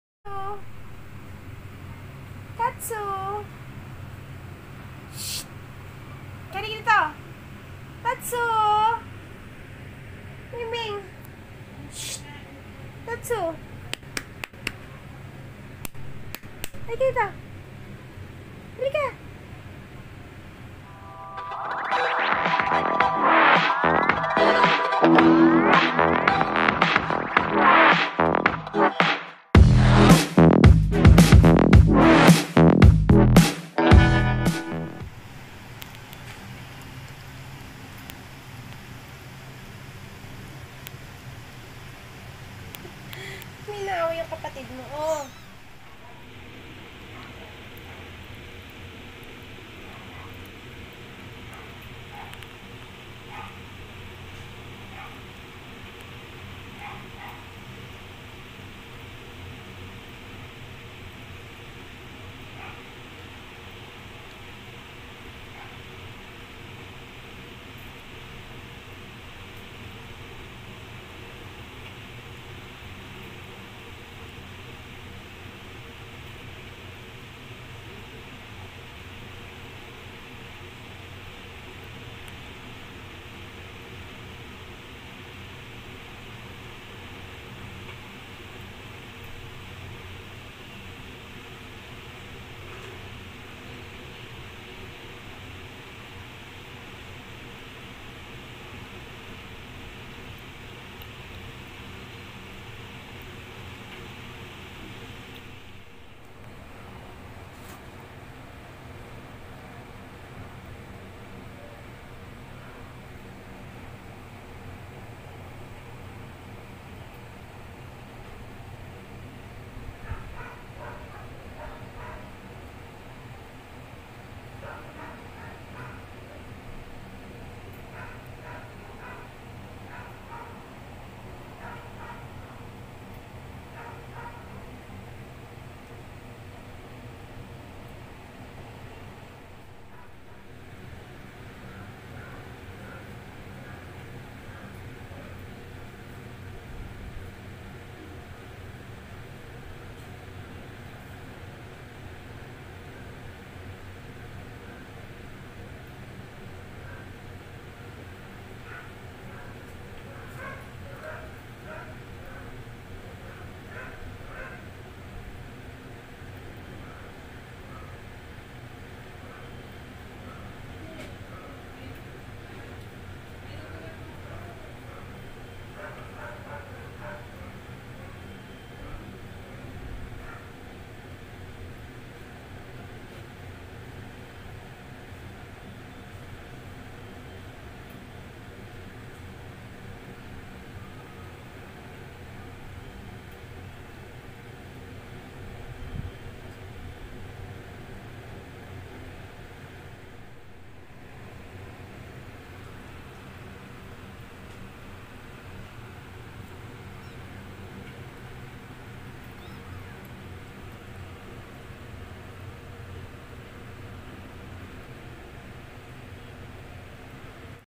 How To feed a Kitten Using a Syringe | Kitten's Feeding Time | SenioritaLynn
Hi I'm Lynn Marfil and this video is all about feeding a kitten using syringe

2. This video is not sponsored and not associated with any company or products mentioned on the video.
3. No intention to infringe photos or audio used in this video.

Kindly follow me at